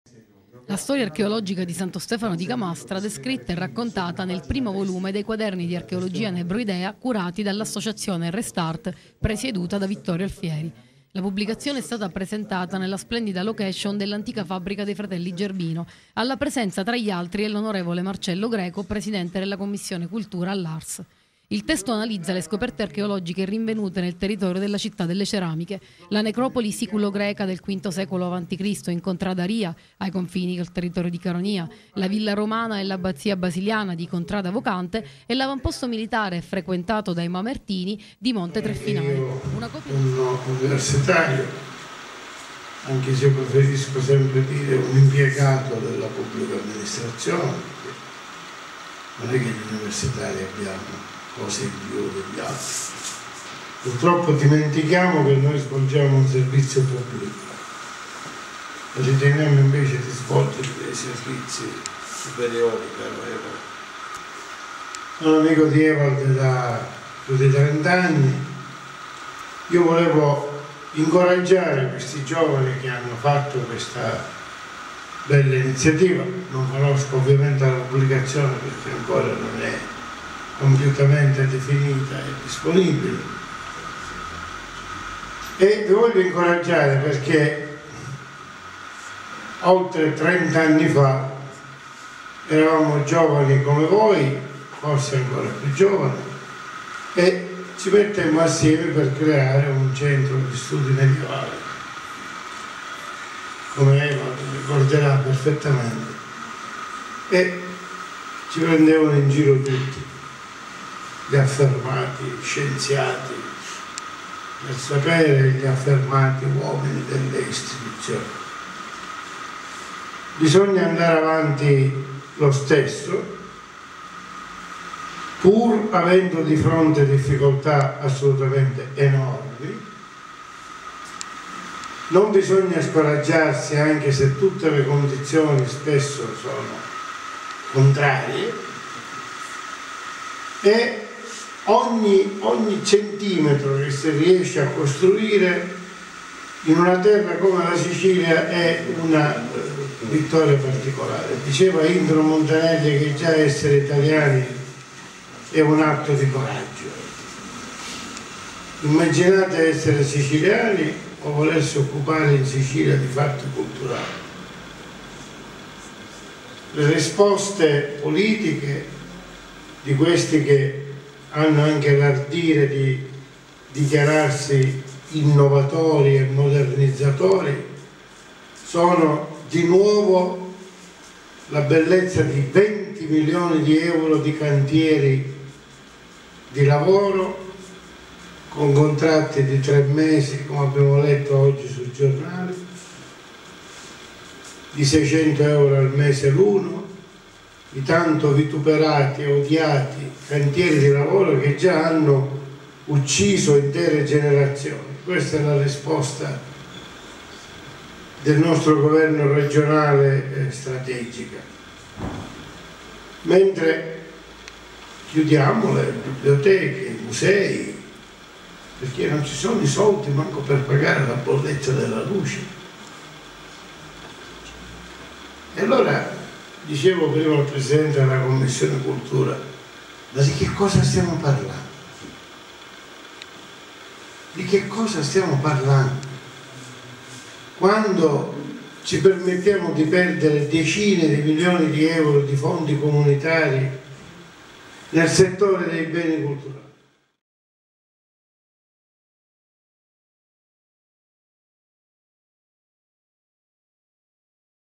Ricordo del compianto Prof. Sandro Musco
A titolo personale e a nome e per conto dell'Associazione Culturale Rest@rt a.p.s., che mi pregio di presiedere e di coordinare scientificamente, pubblico un breve stralcio dell'intervento che il Prof. Alessandro Musco ci ha onorato di fare alla conferenza stampa indetta il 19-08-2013 per la presentazione alla comunità scientifica internazionale della pubblicazione a carattere archeologico intitolata "Santo Stefano di Camastra - Notebooks of Nebroidean Archaeology".
In quell'occasione - come del resto in tutte le altre, sia pubbliche che private, in cui io ho avuto il piacere di intrattenermi con Lui - il Prof. Alessandro Musco ha dimostrato di avere una onestà intellettuale, una preparazione professionale ed un carisma e un calore umano fuori dal comune!
Sono orgoglioso ed emozionato di aver goduto della Sua stima e della Sua approvazione!